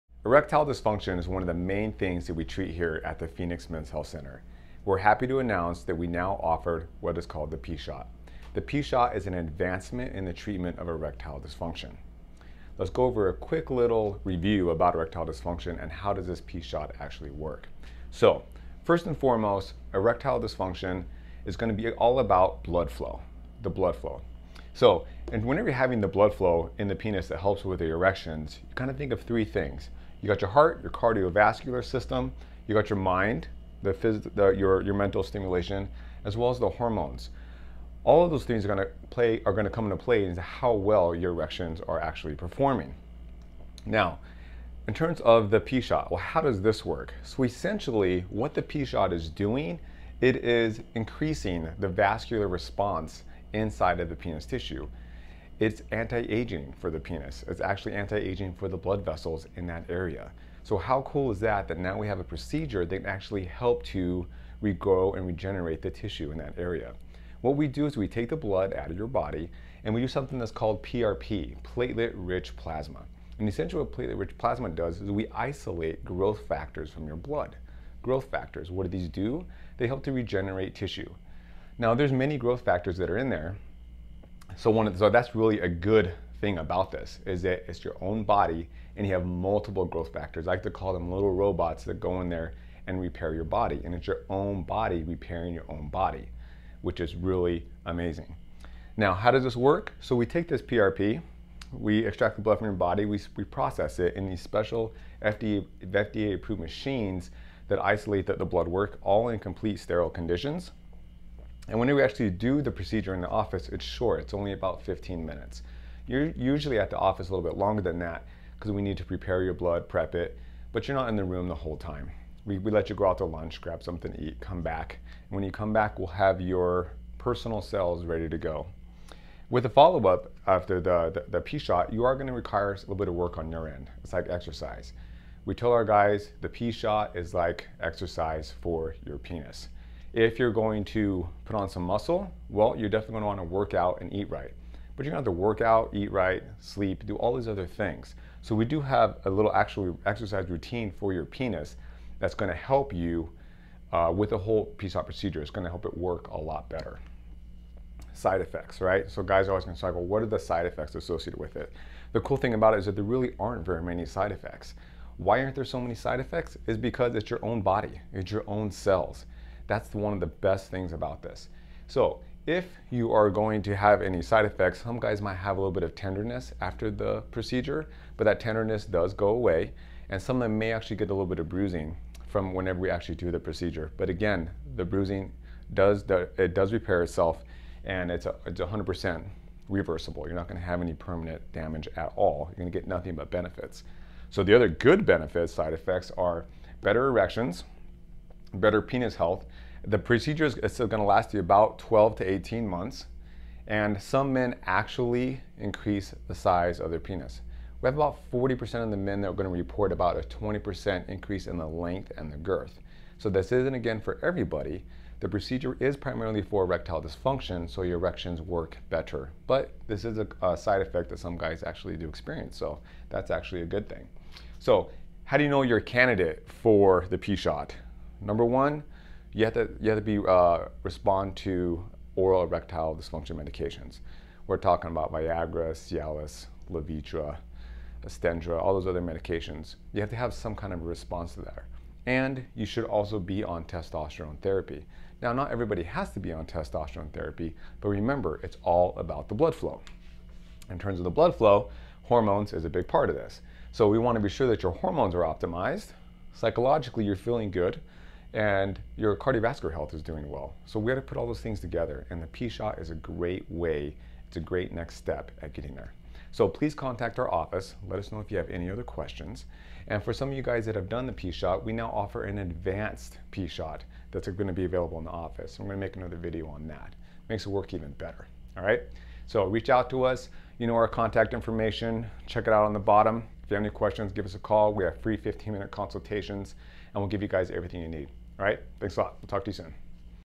PShot w: Dr Le Provost
Dr. Le Provost gives us a quick break down on erectile dysfunction (common factors)and how The Priapus Shot(P-Shot) works and how it is prepared.